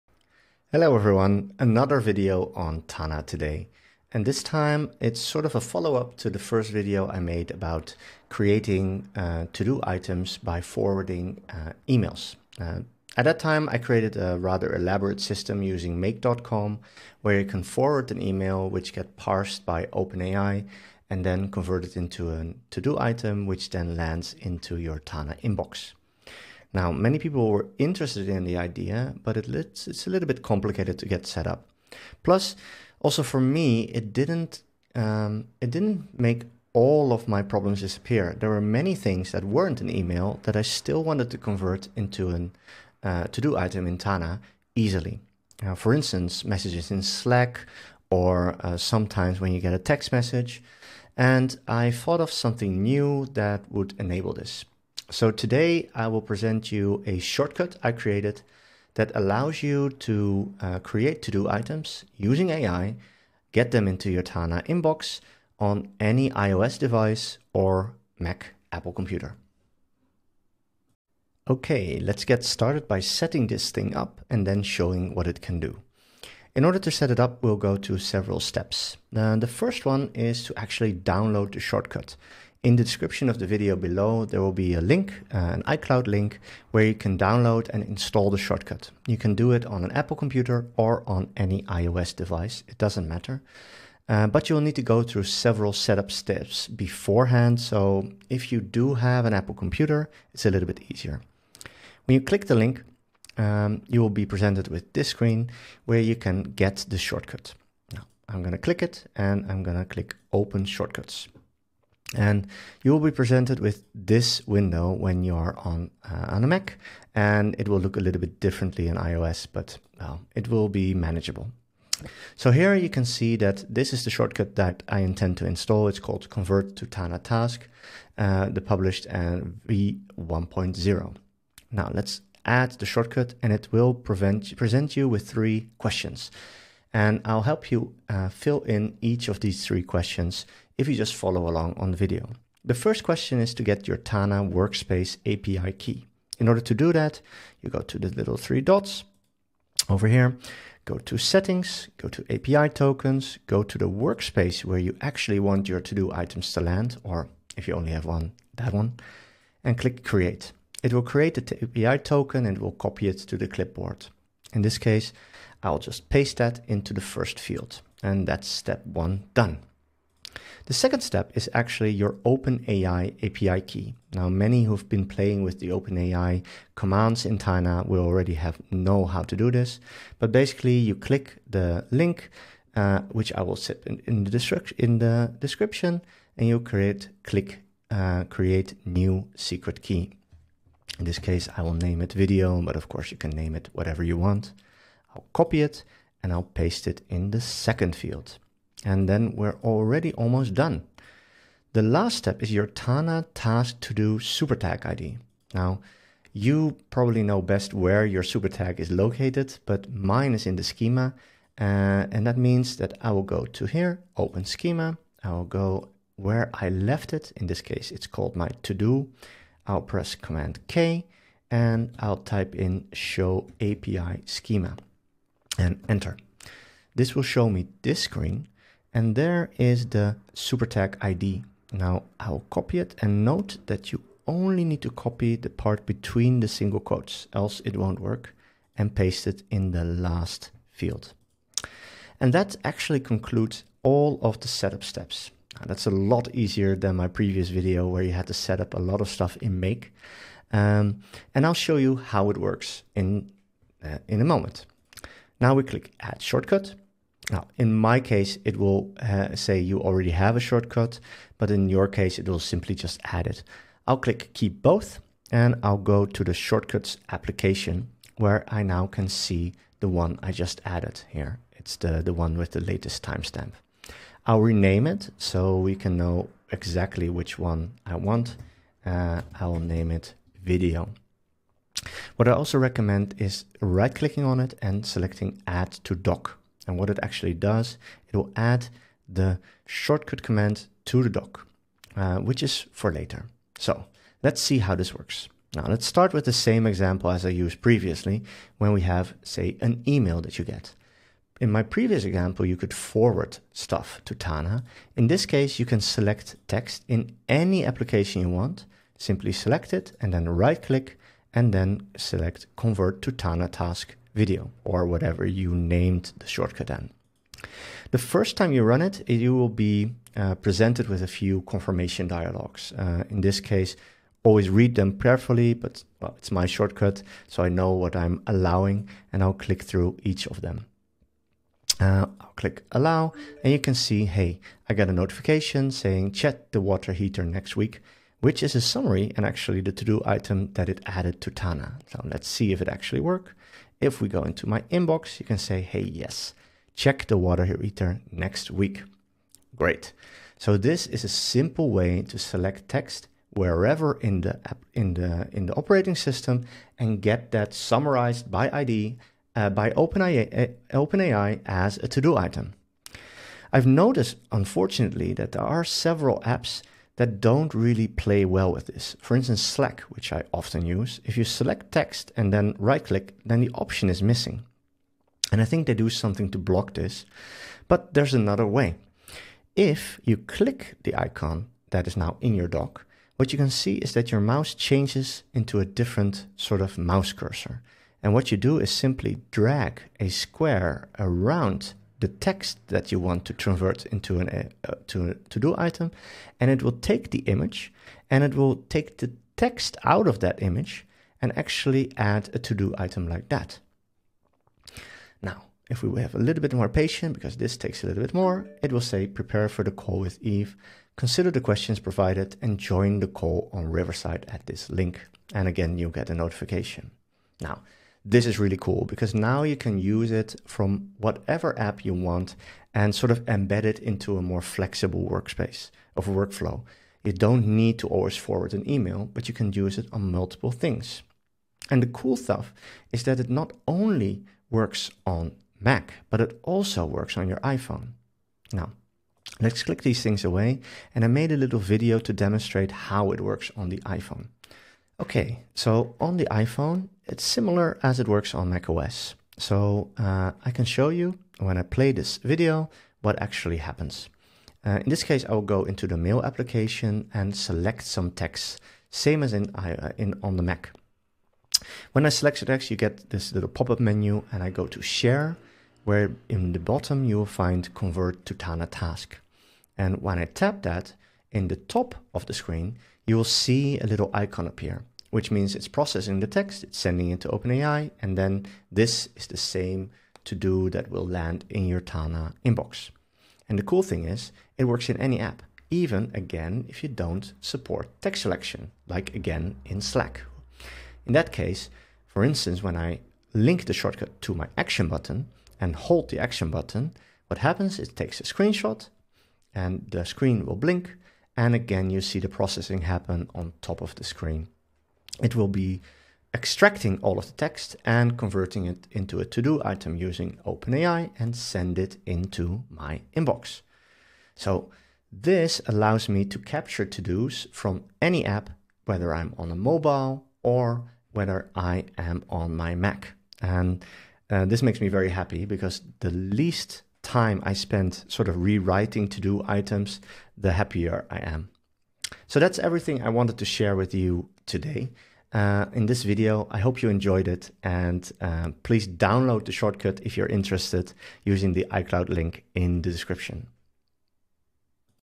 Using AI to add todo items to Tana from any app on iOS and MacOS
In this video I show how to use my Shortcut to use OpenAI to convert any text to a todo item and add it to the Tana inbox. It works on both iOS and MacOS.